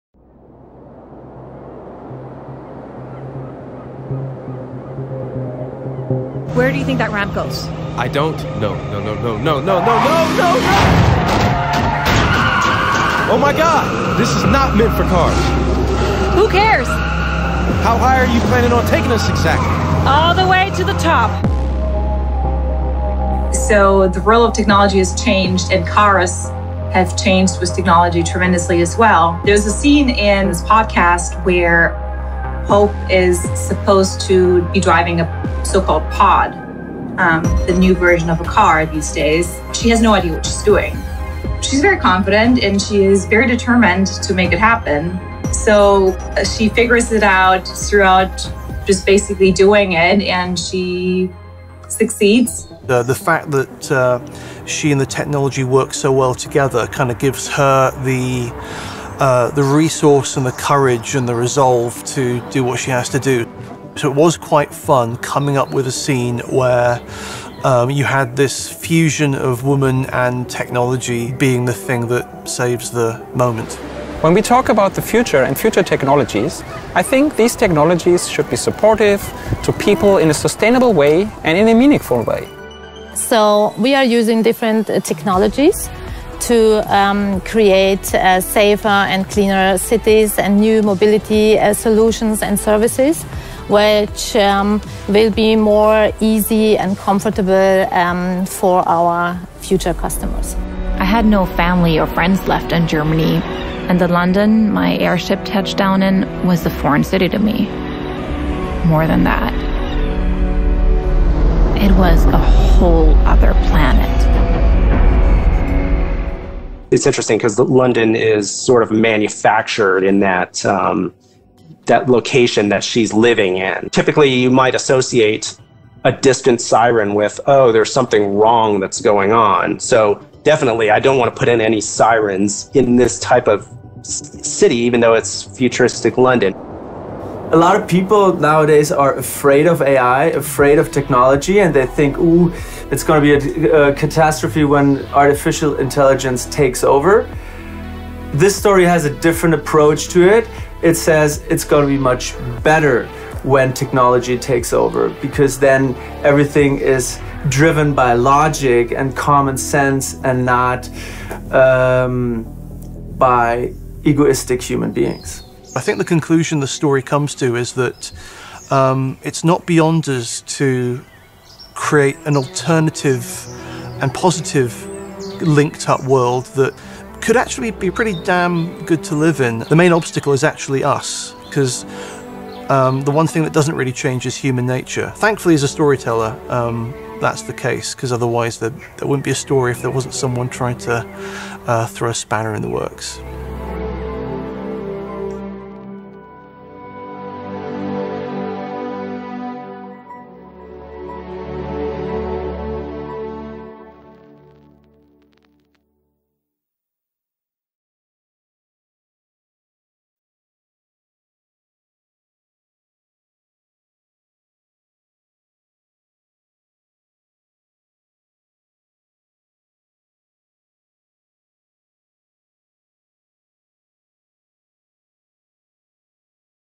HYPNOPOLIS | A vision of the future
Despite all the dangers that the heroine Hope Reiser encounters on her adventure, the science fiction thriller HYPNOPOLIS paints a positive picture of the future. Charlotte Taschen, the voice artist for Hope Reiser, and author Robert Valentine explain the role of mobility using the example of a high-speed chase. Also, in part three of our mini-documentary: Zana Koval, Head of Sales and Marketing BMW Welt, shows us her workplace and sound designer Michael Ferdie talks about the sound of metropolises in the year 2063.